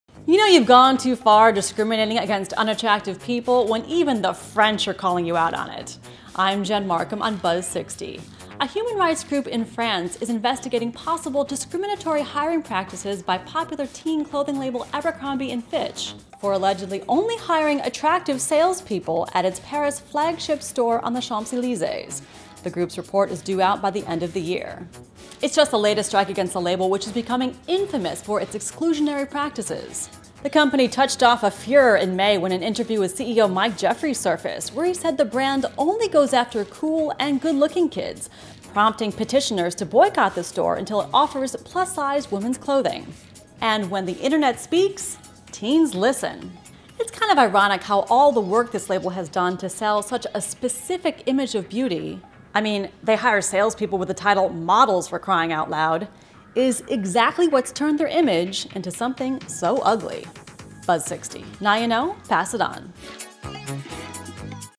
Abercrombie & Fitch Accused of Hiring Based on Looks in France
A human rights group in France is investigating possible discriminatory hiring practices by popular teen clothing brand Abercrombie & Fitch, for allegedly hiring people based off looks at its Paris flagship store. Jen Markham explains why this is just the latest strike against a clothing label developing an ugly reputation for its standards of beauty.